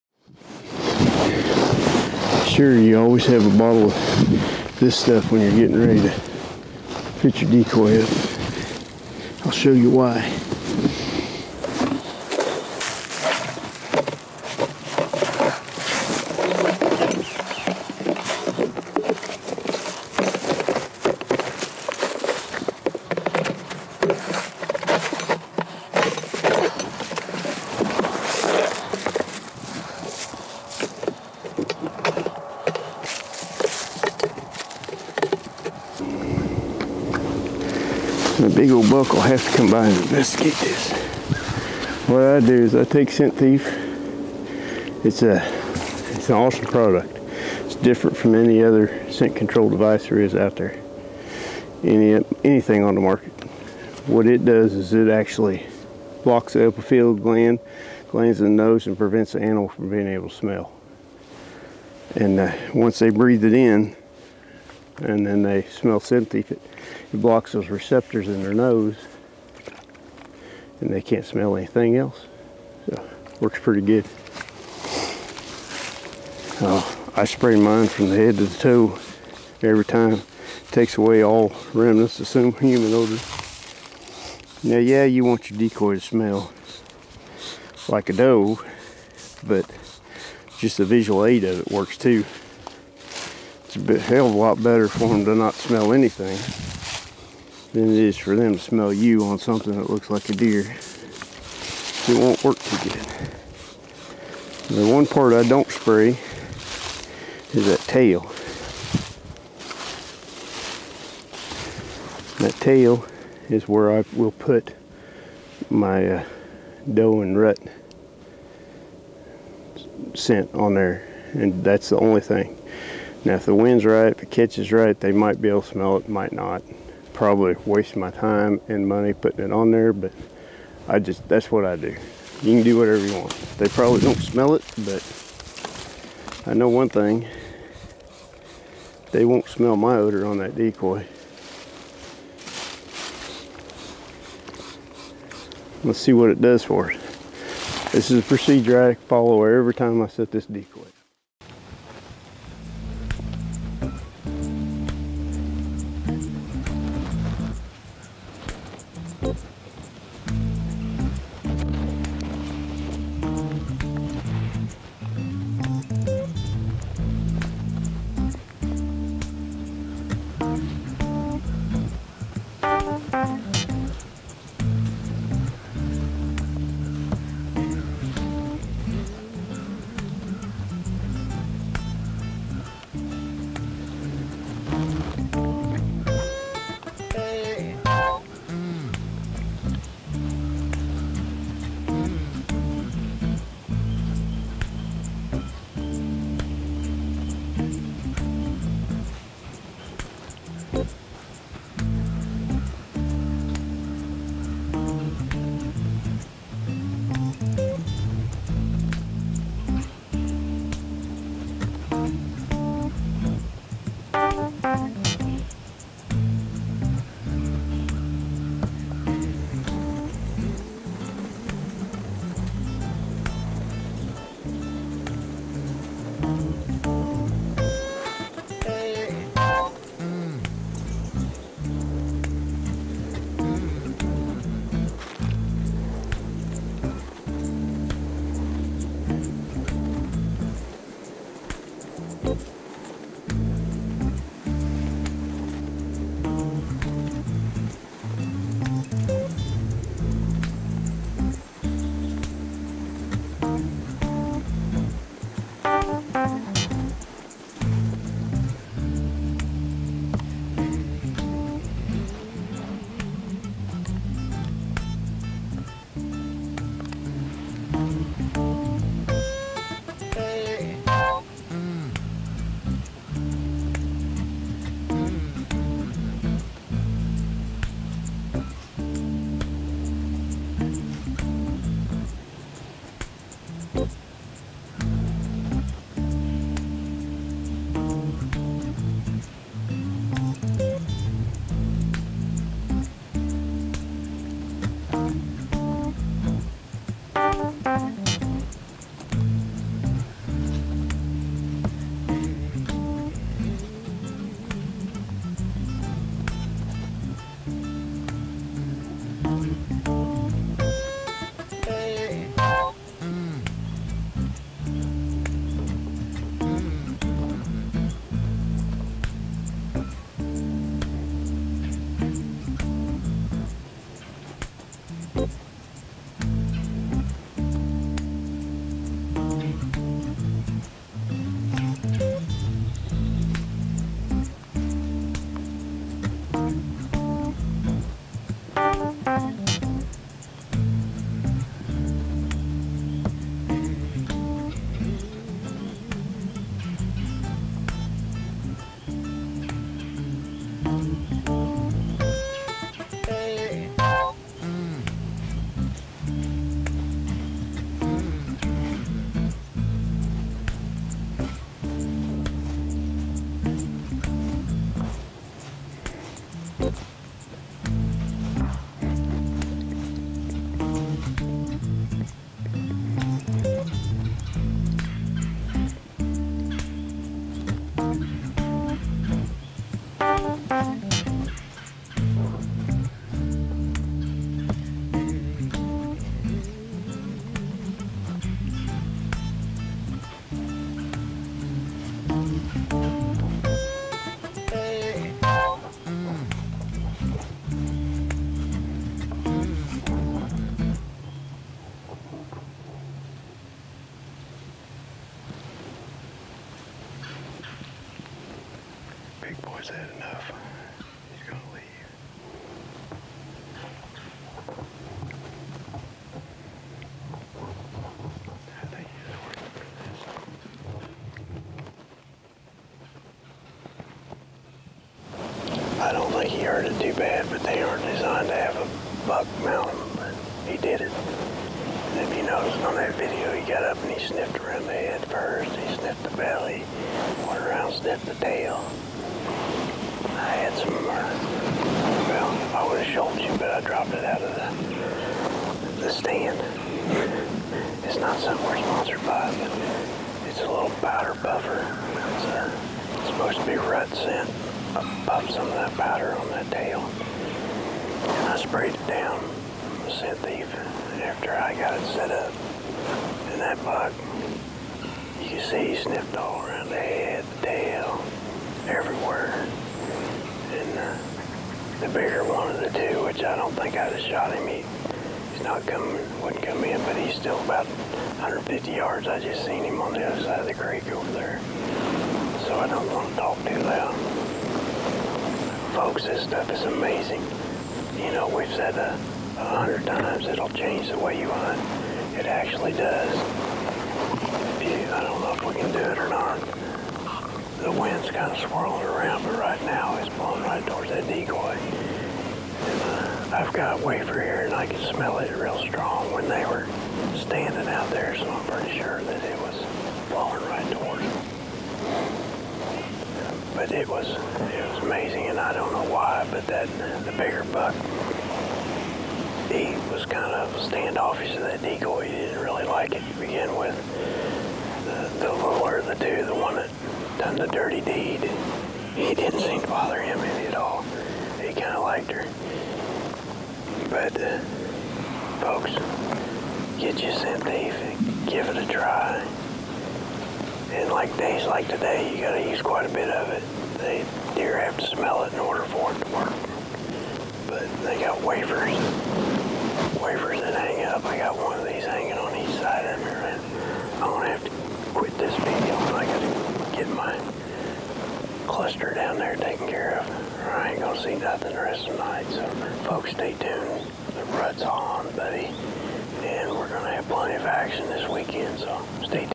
Decoy things go too far
Watch as this decoy gets a lot more then it was designed for. This buck took things a little too far. After she falls to the ground he tries to stand her back up to finish.